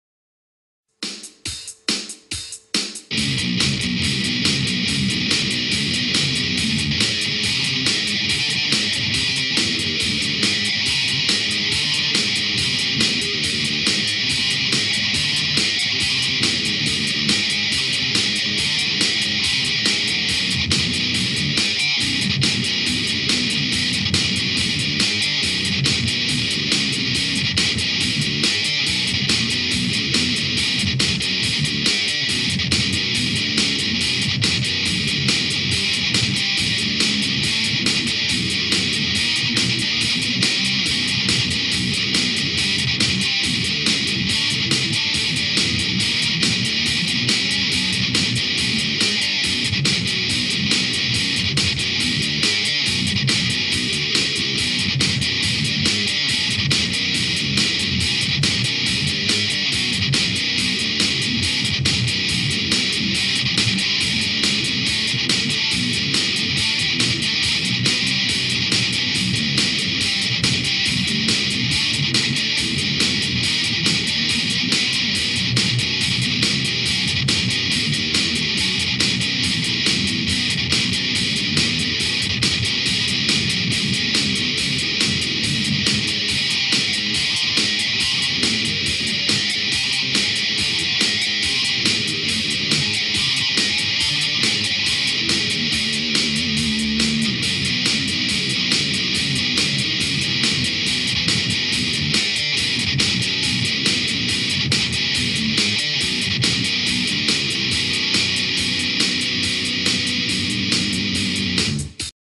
Remix of a metal song idea I had back in 2009..
REMIX
Writing arranging practicing - New heavy metal song - Original music Gary Hutchings

I play a 2000 Ibanez RG7421 7-String Electric Guitar through a Digitech GNX2 genetx guitar processor into a Nady PMX-1600 is a 16 Channel 4 Bus Powered Mixer